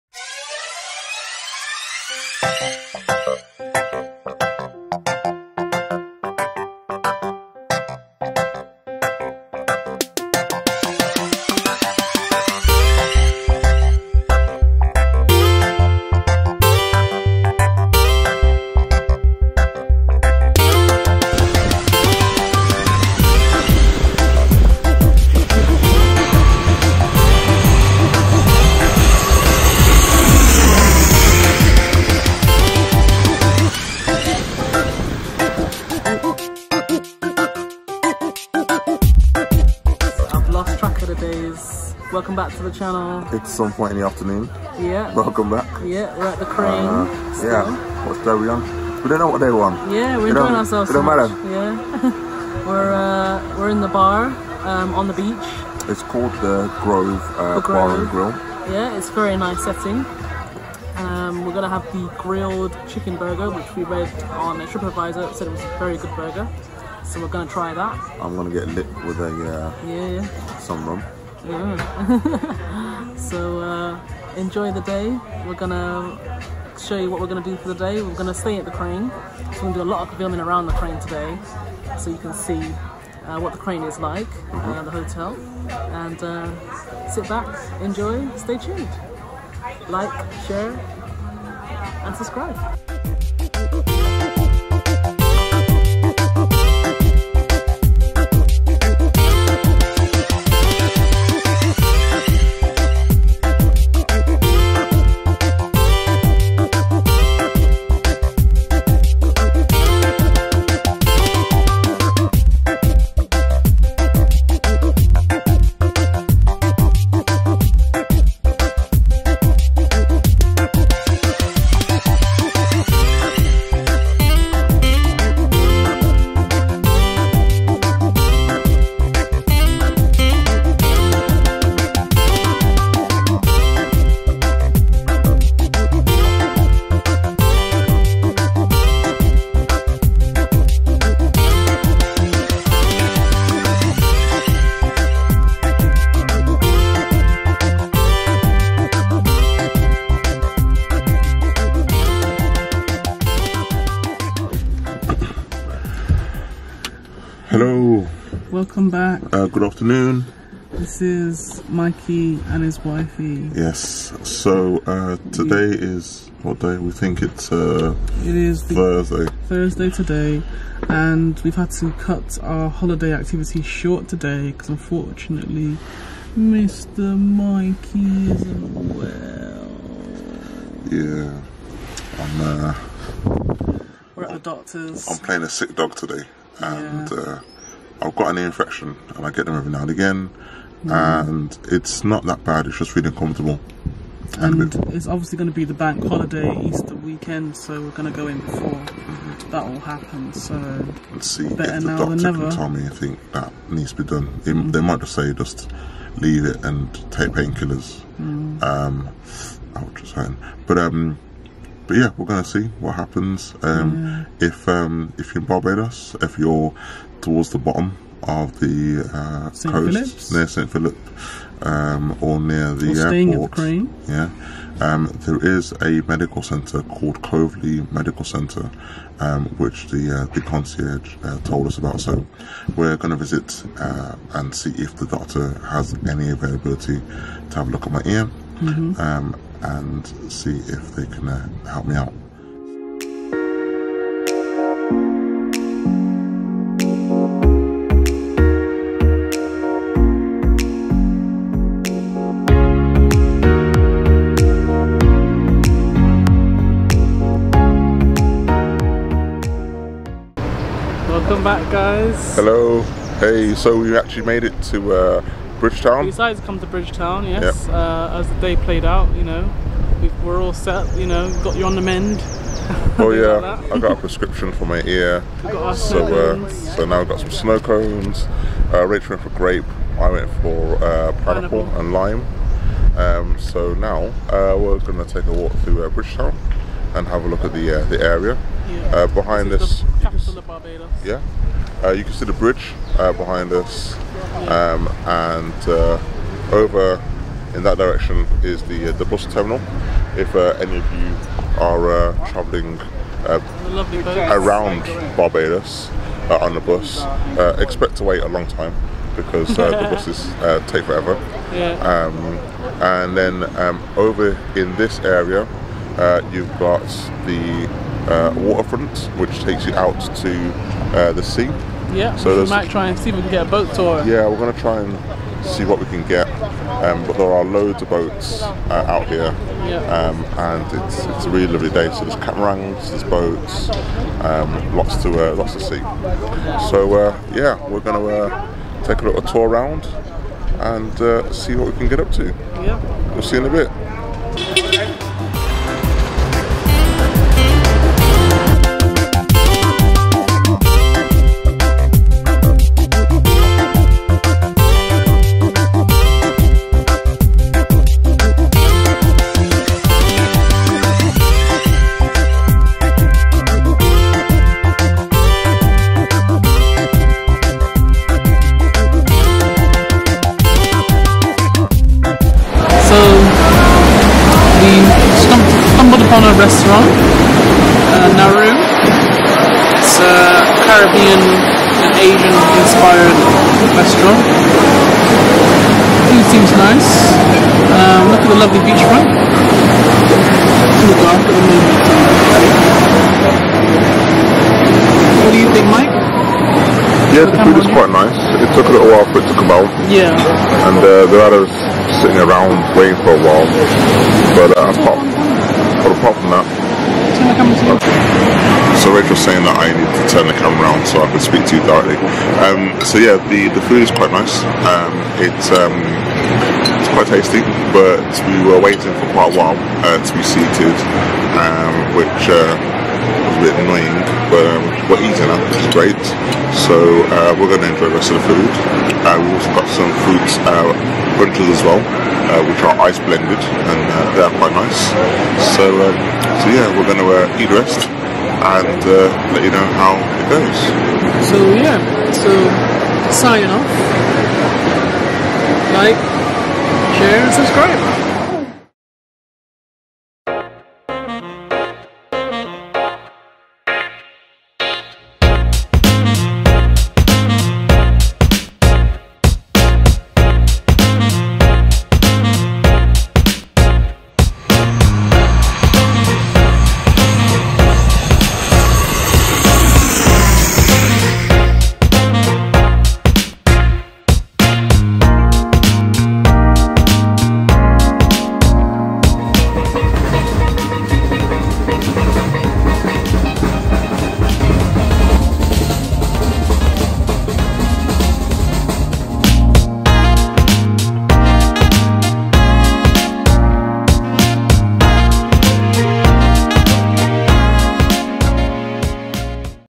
Barbados Beach Bars & Bridgetown
Welcome to Mikey & His Wifey’s Vlog Channel. We are a couple from the UK, loving life geeky chic family style.

In this Vlog we visit the capital of Barbados Bridgetown, and explore the beach bar at The Crane resort